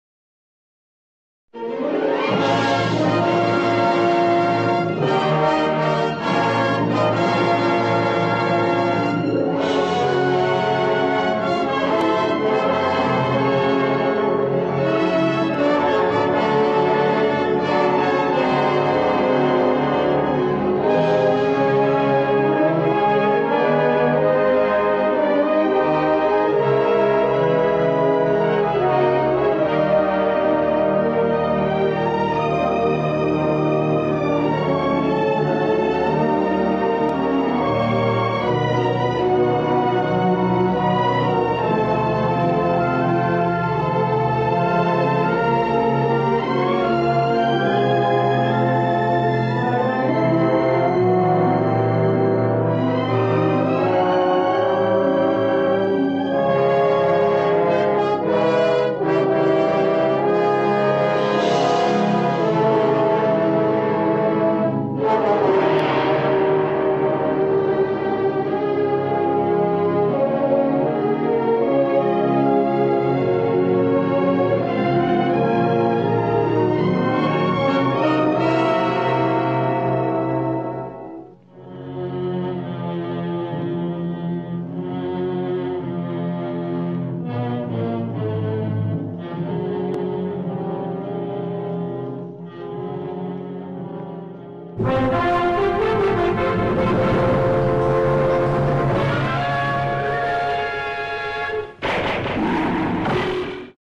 Childhood Movie GREATS
Nostalgia flashback: Classic motion picture blockbusters I could catch via tv-reruns as a kid; maybe bringing back some good memories for You, too.